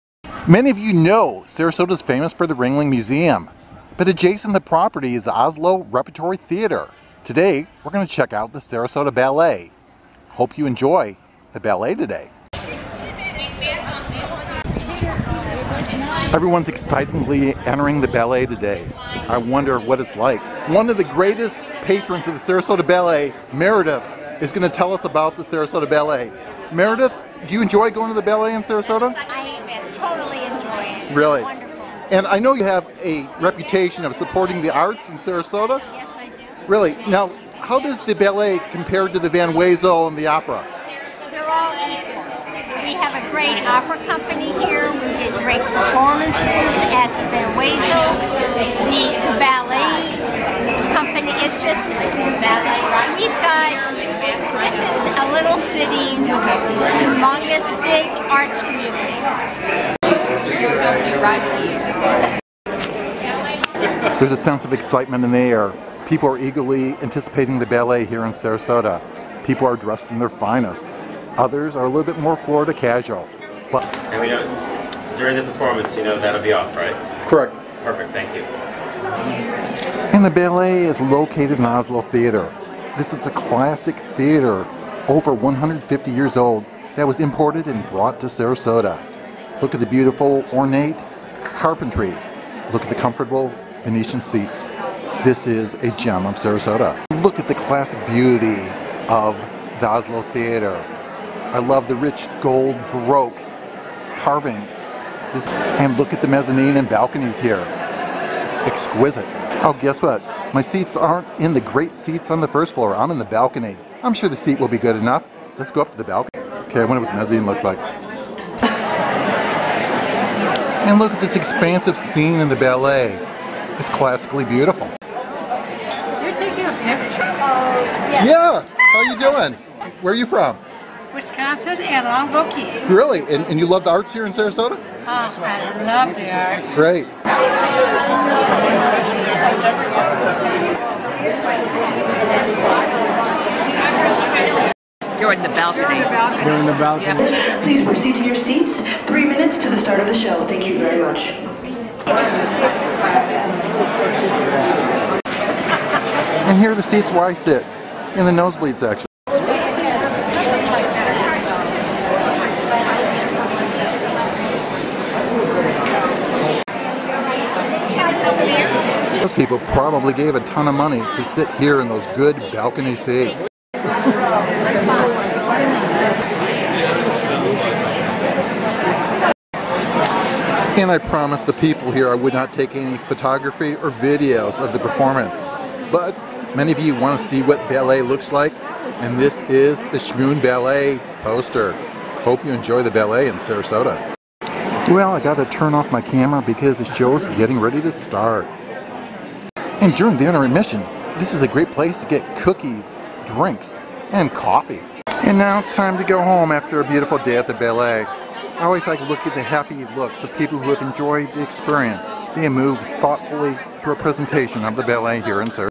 Experience the Magic of Ballet at the Sarasota Ballet 🩰✨
Join us as we delve into the enchanting world of ballet at the renowned Sarasota Ballet in Sarasota, Florida 🩰✨. In this video, we'll take you on a captivating journey through the mesmerizing performances, elegant choreography, and talented dancers that make the Sarasota Ballet a popular cultural activity in the area. So, gather your friends and family, and let's experience the magic of ballet at the Sarasota Ballet.